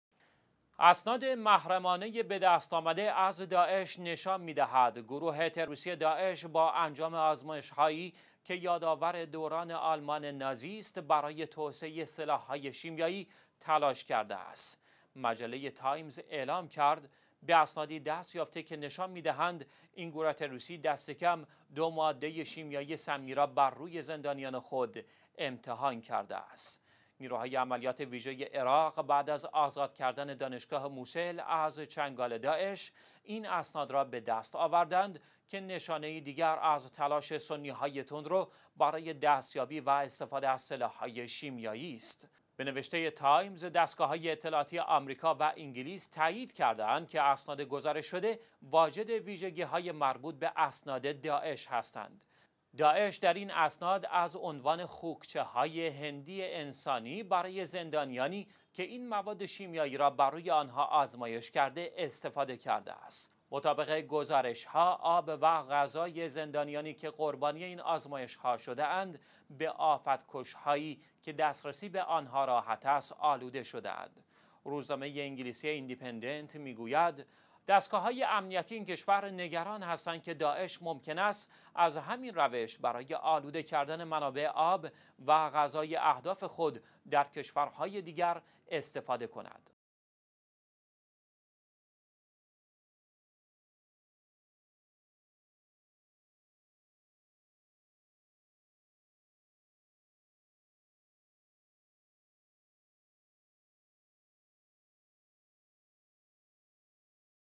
اسناد جنایت های هولناک سنى های تندروى داعش، در عراق
اسناد محرمانه به دست آمده از داعش نشان می‌دهند، گروه تروریستی داعش با انجام آزمایش‌هایی که یادآور دوران آلمان نازی است، برای توسعه سلاح‌ های شیمیایی تلاش کرده است.

مجله تایمز اعلام کرد به اسنادی دست یافته که نشان می‌دهند این گروه تروریستی دست کم دو ماده شیمیایی سمی را روی زندانیان خود امتحان کرده است.

نیروهای عملیات ویژه عراق، بعد از آزاد کردن دانشگاه موصل از چنگال داعش، این اسناد را به دست آورده‌اند، که نشانه ای دیگر از تلاش سنى هاى تندرو برای دست‌یابی و استفاده از سلاح‌های شيميايي است.

به نوشته تایمز، دستگاه‌های اطلاعاتی آمریکا و انگلیس تأیید کرده‌اند که اسناد گزارش‌شده، واجد ویژگی‌های مربوط به اسناد داعش هستند. داعش در این اسناد از عنوان «خوکچه‌های هندی انسانی» برای زندانیانی که این مواد شیمیایی را بر روی آن ها آزمایش کرده؛ استفاده کرده است.

مطابق گزارش‌ها، آب و غذای زندانیانی که قربانی این آزمایش ها شده‌اند، به آفت‌کش‌هایی که دسترسی به آن ها راحت است، آلوده شده‌اند.

روزنامه انگلیسی «ایندیپندنت» می‌گوید: دستگاه‌های امنیتی این کشور نگران هستند که داعش ممکن است از همین روش برای آلوده کردن منابع آب و غذای اهداف خود در کشورهای دیگر، استفاده کند.

سه شنبه - ۲ خرداد ۱۳۹۶
الثلاثاء - ٢٦ شعبان ١٤٣٨